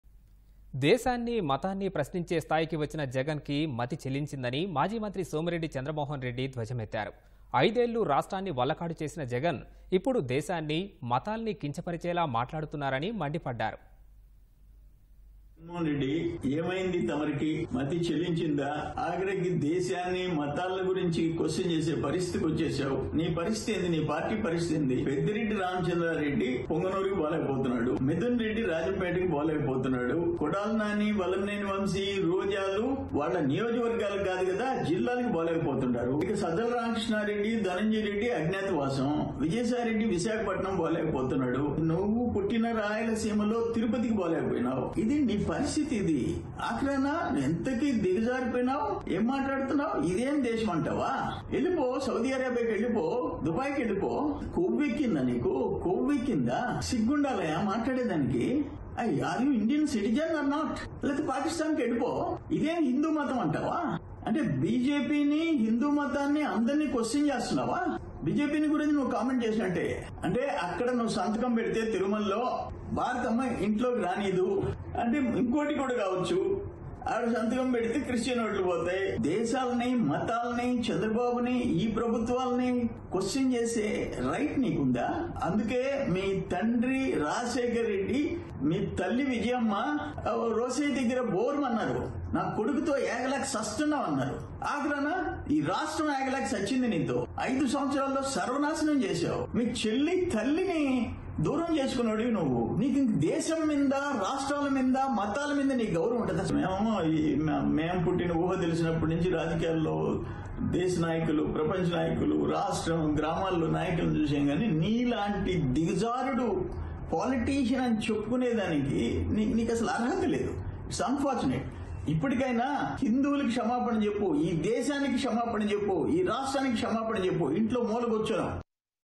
జగన్‌..మతి చెలించిందా..? దేశాన్ని ప్రశ్నిస్తున్నావు | సోమిరెడ్డి | Jagan Declaration Row
దేశాన్ని, మతాన్ని ప్రశ్నించే స్థాయికి వచ్చిన జగన్ కి మతి చెలించిందని... మాజీ మంత్రి సోమిరెడ్డి చంద్రమోహన్ రెడ్డి ధ్వజమెత్తారు. ఐదేళ్లు రాష్ట్రాన్ని వల్లకాడు చేసిన జగన్ ... ఇప్పుడు దేశాన్ని, మతాల్ని కించపరిచేలా మాట్లాడుతున్నారని. మండిపడ్డారు.